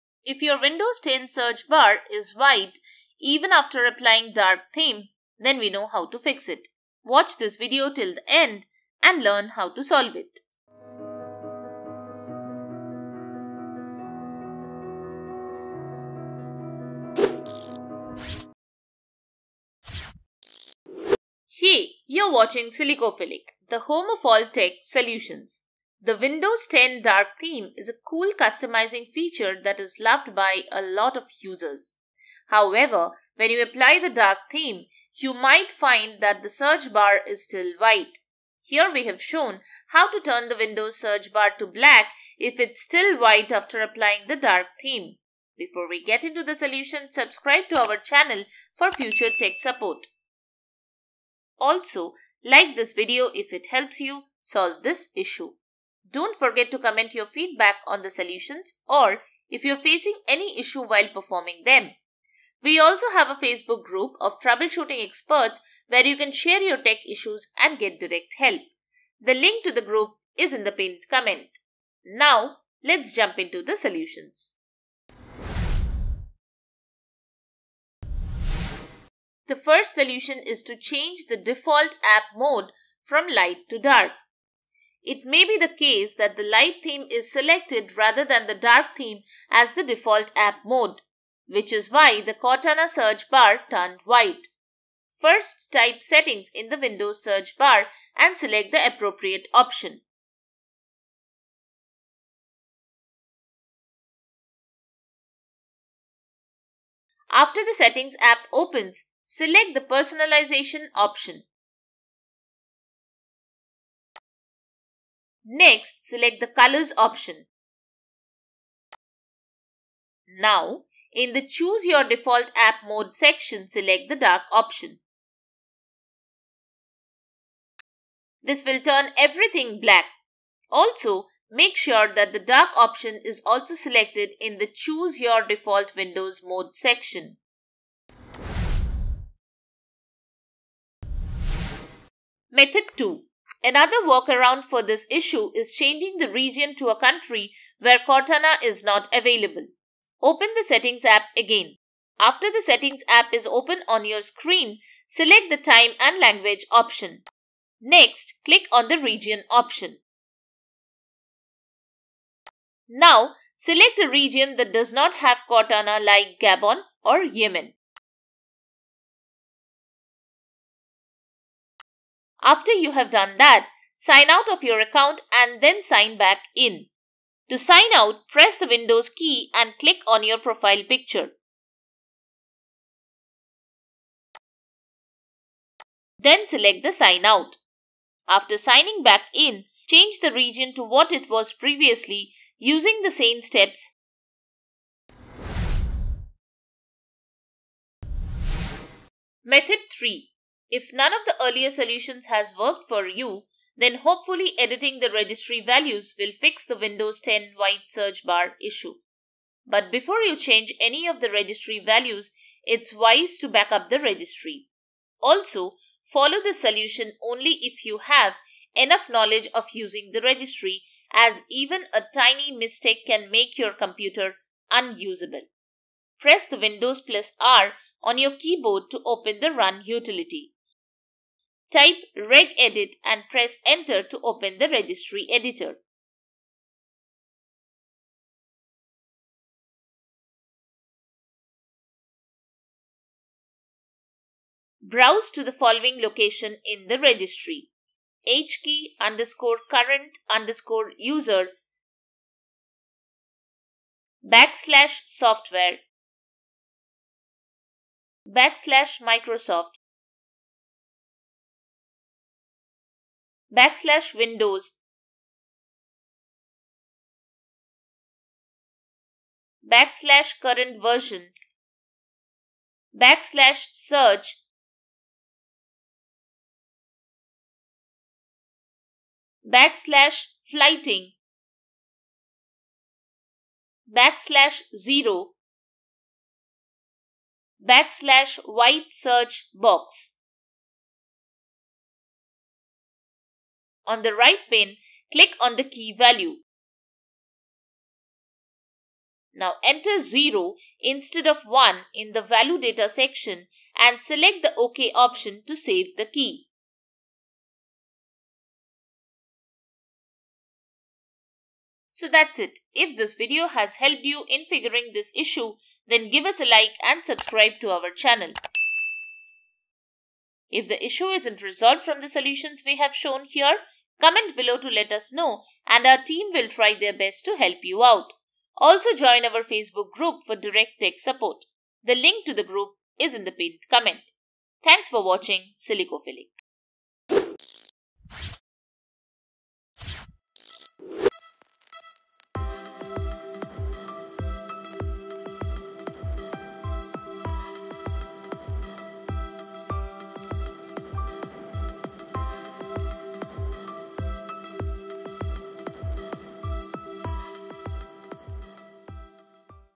How to FIX The WHITE SEARCH BAR in Windows 10 After Applying a DARK Theme?
If you've applied a dark theme to Windows 10 and noticed that the search bar is still white, don't worry, just follow the steps in this video to fix the white search bar in windows 10 after applying a dark theme.

Registry Path: HKEY_CURRENT_USER\Software\Microsoft\Windows\CurrentVersion\Search\Flighting\0\WhiteSearchBox

Timestamps:
00:00 Intro
01:17 Change the Default App Mode
02:20 Change the Region
03:17 Modify Registry